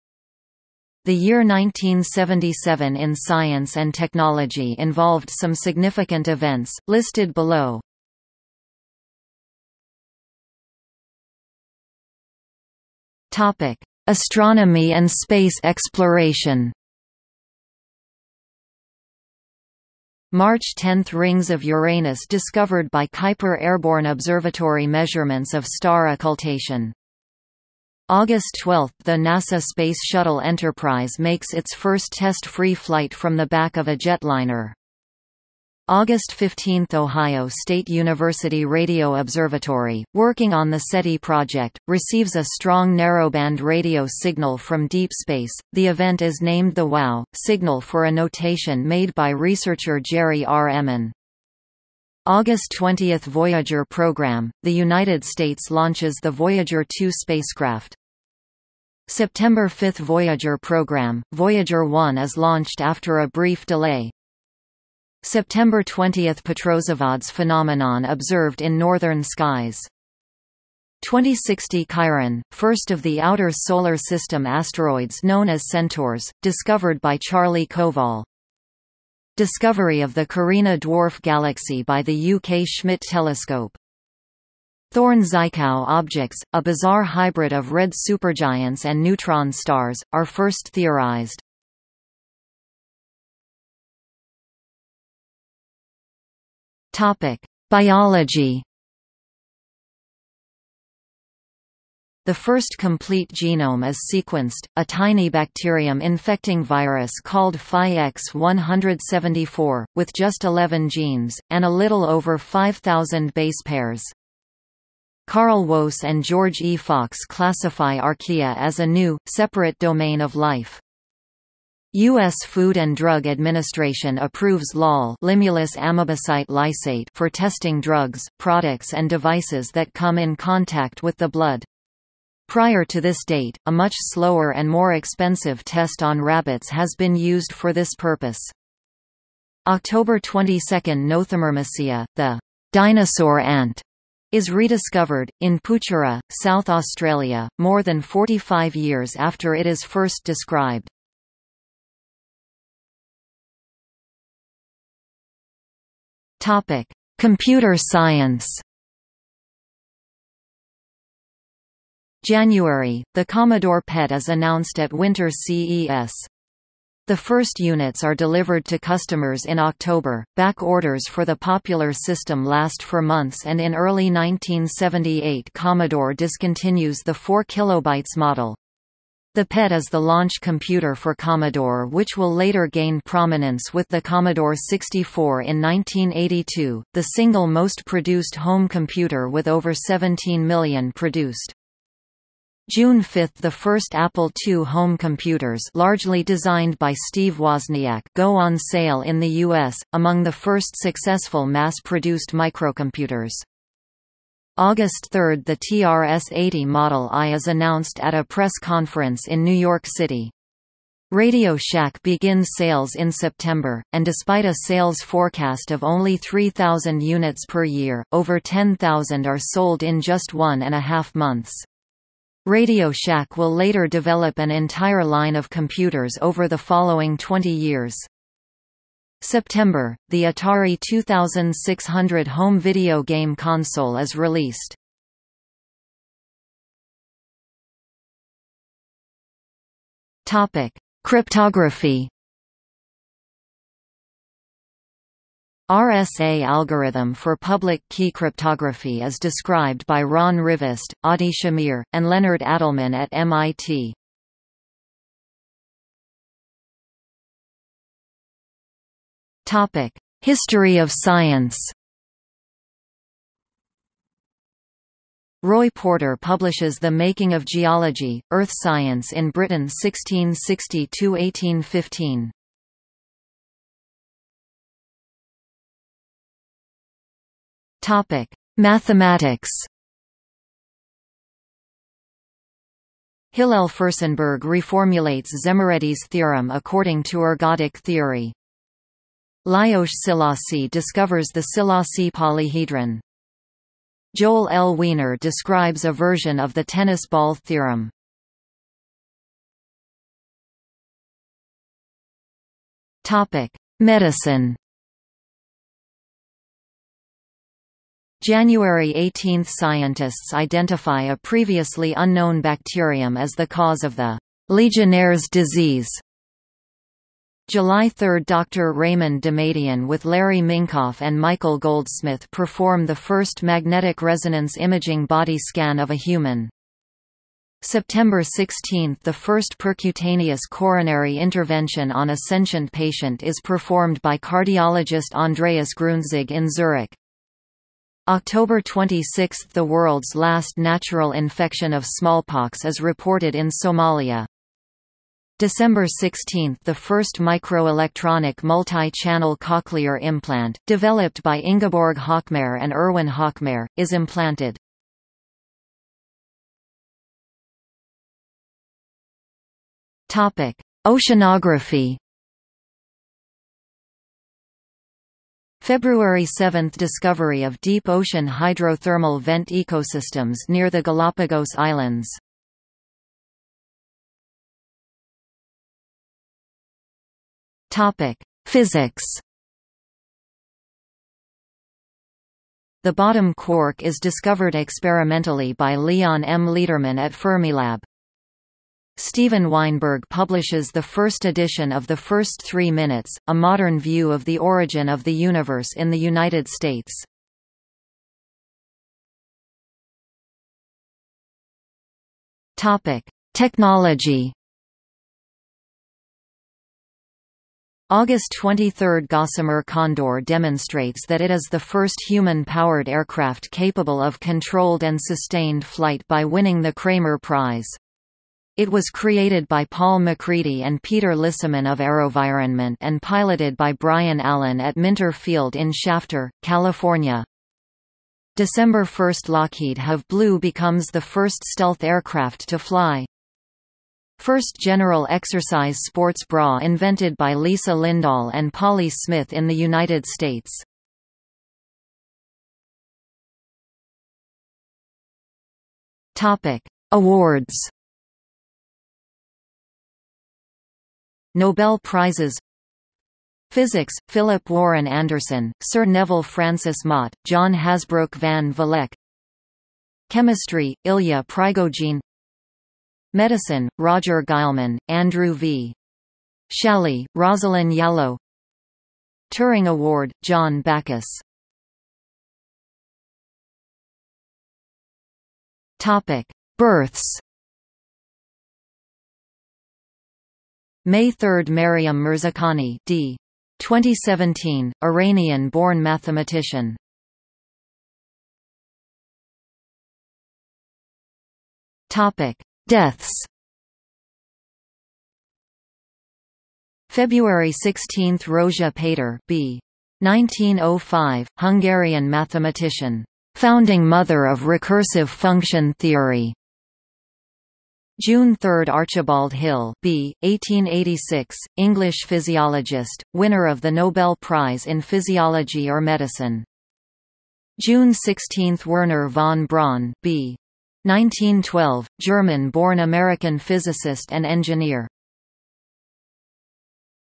1977 in science | Wikipedia audio article
00:00:09 1 Astronomy and space exploration
00:01:33 2 Biology
00:02:32 3 Computer science
00:03:56 4 Cryptography
00:04:15 5 History of science
00:04:34 6 Mathematics
00:04:59 7 Medicine
00:05:56 8 Oceanography
00:06:14 9 Physics
00:06:39 10 Technology
00:07:22 11 Awards
00:07:52 12 Births
00:08:08 13 Deaths

- Socrates